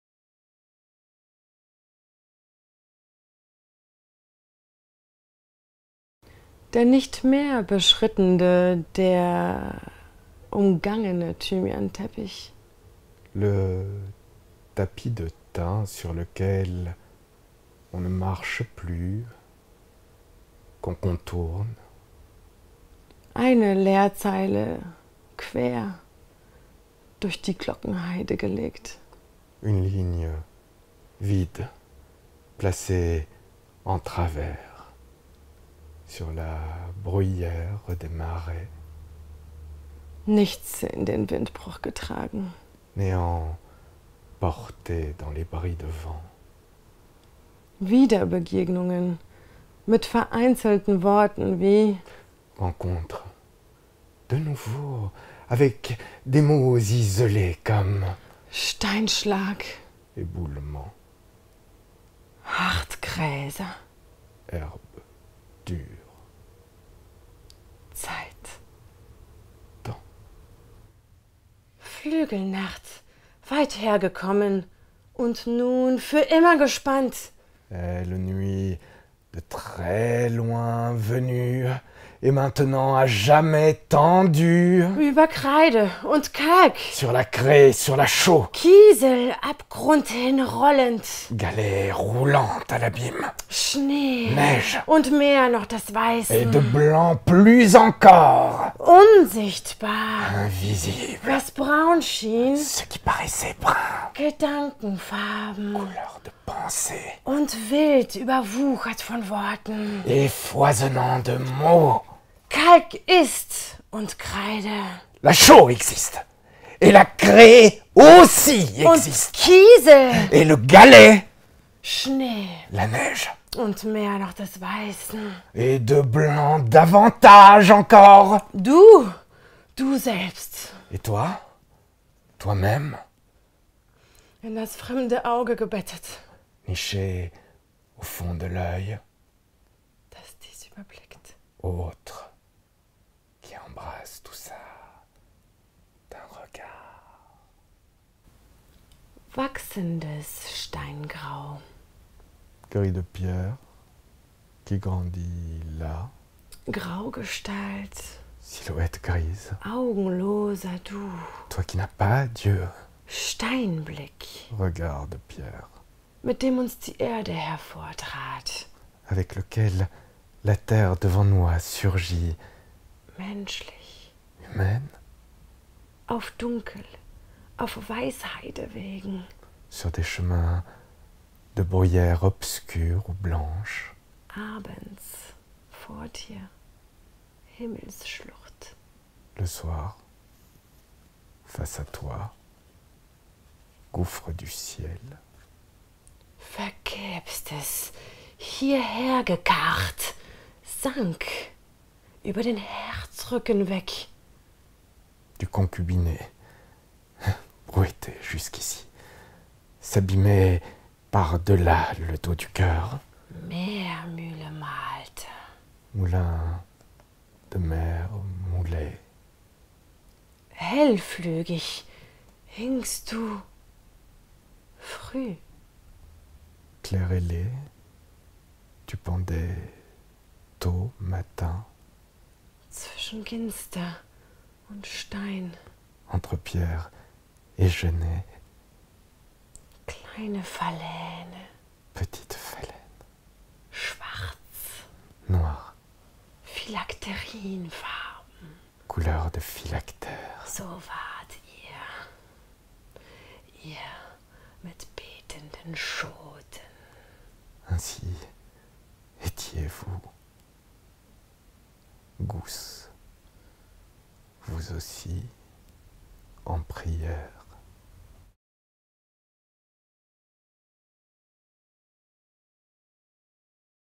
Paul Celan: ''Choix de poèmes'', Gallimard, 2020
Lecture par Sonja Kropidłowska & Lysztéria Valner